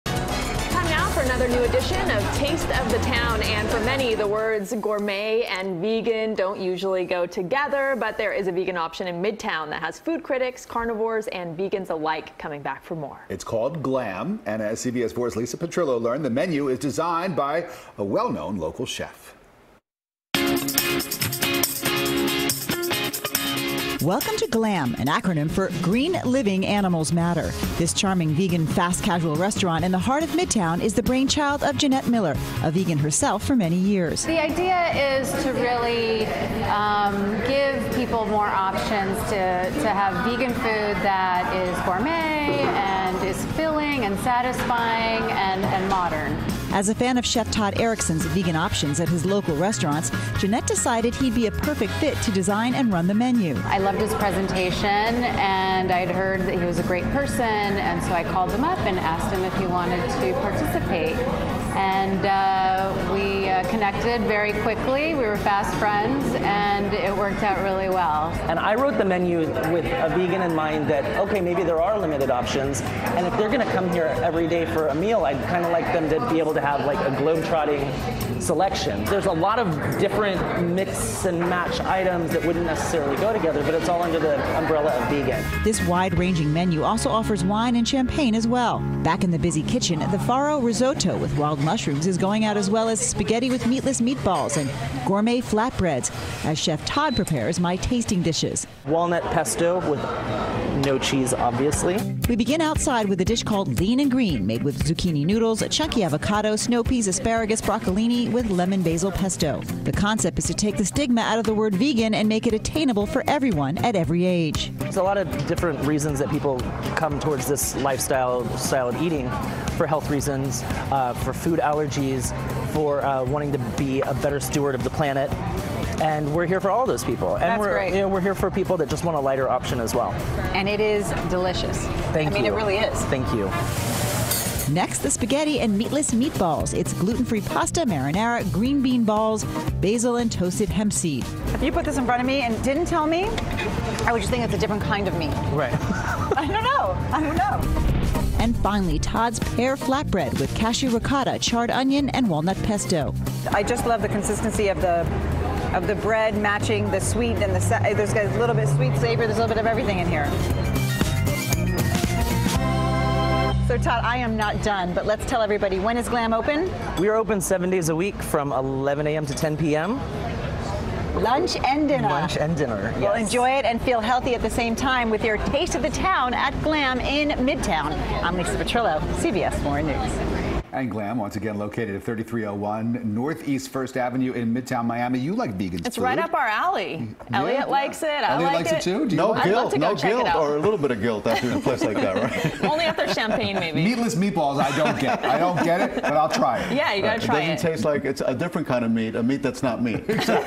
Taste Of The Town: GLAM Gives Midtown Vegans Plenty To Chew On
CBS Miami’s Lisa Petrillo reports.